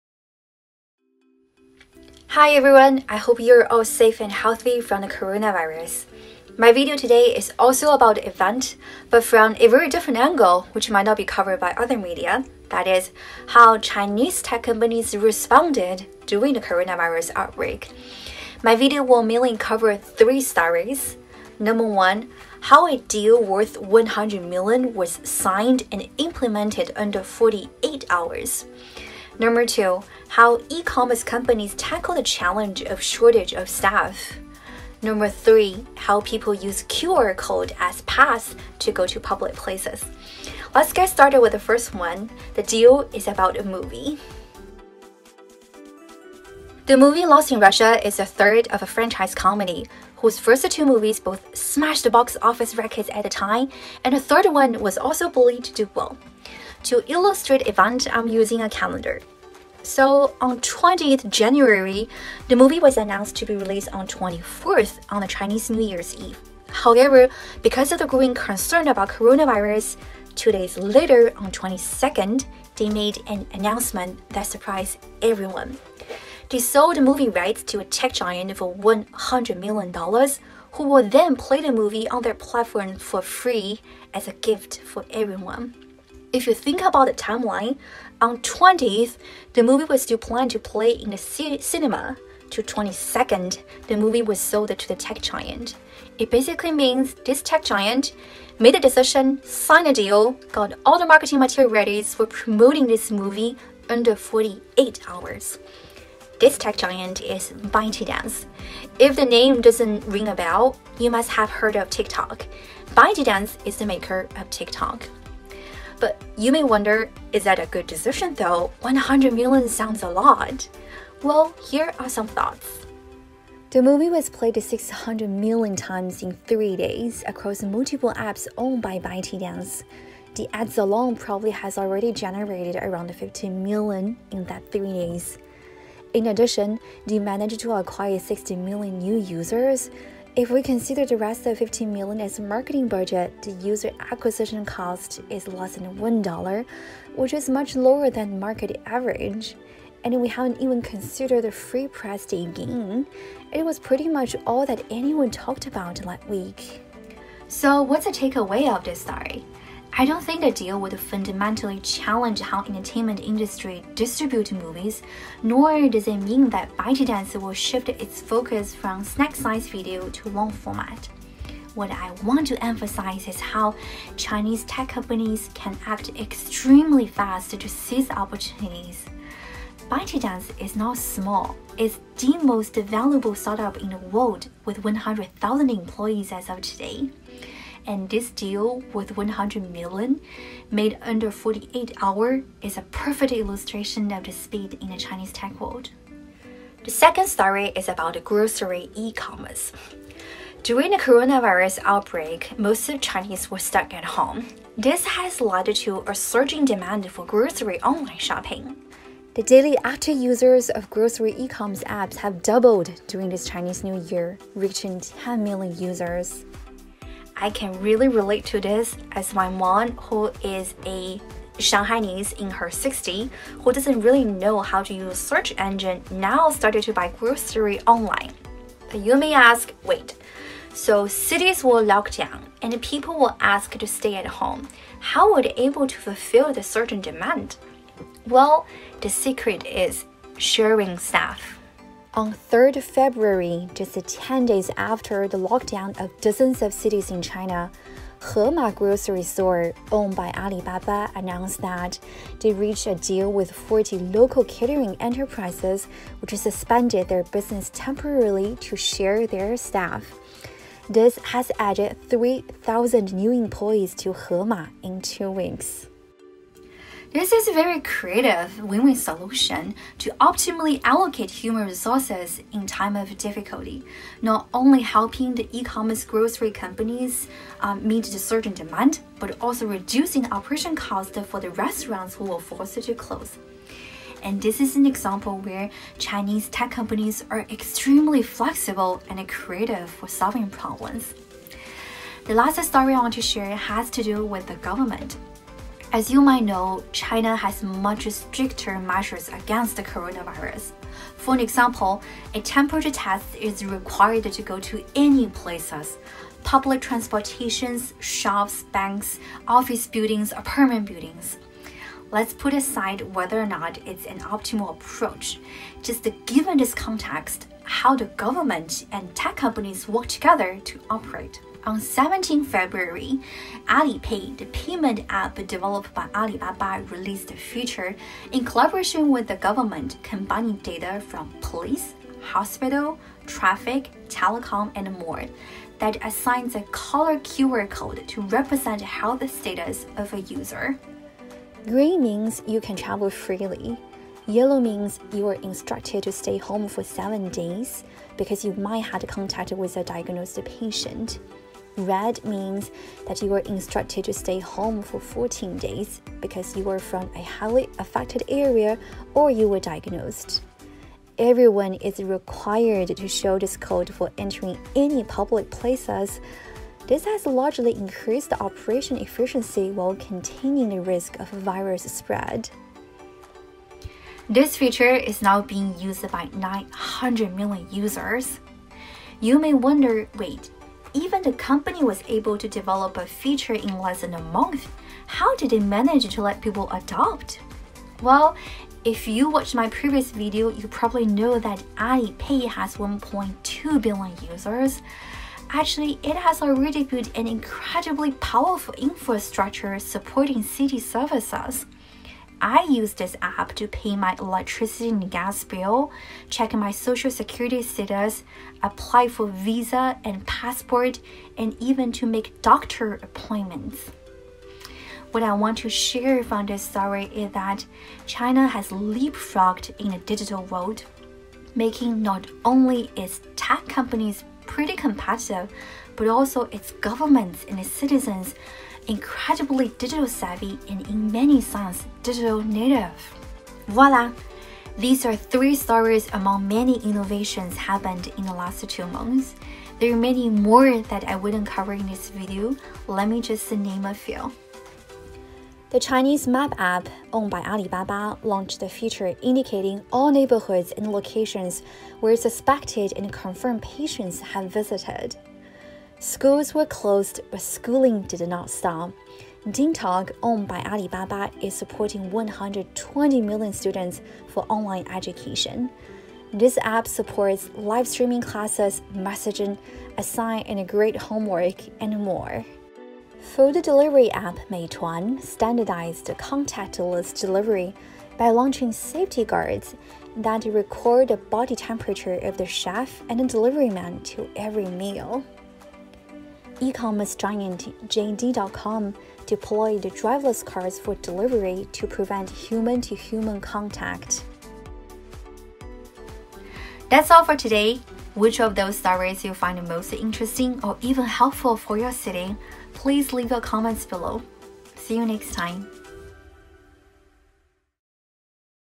Chin Tech Innovations in the battle against Covid-19
0:00 3 stories about how chinese tech companies responded to Coronavirus.
0:40 How a deal worth of $100M was signed an implemented in 48h
3:15 How grocery E-commerce companies tackled the challenge of understaffing
4:51 How QR code is designed to serve as a pass for going to public places